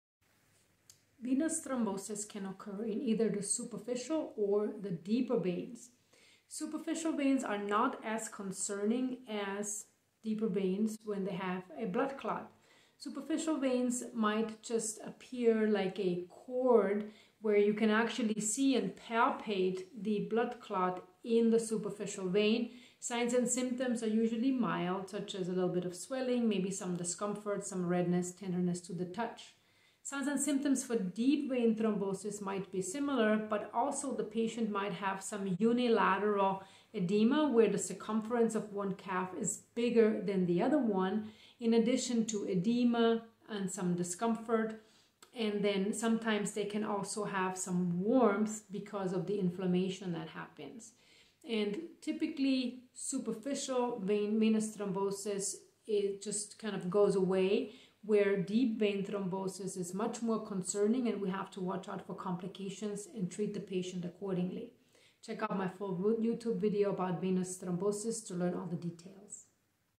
Superficial vs. Deep Vein Thrombosis
This video explains the difference between superficial and deep vein throbmosis.

You may also find these other videos in my CARDIAC DISORDERS and CRITICAL CARE PLAYLISTS helpful: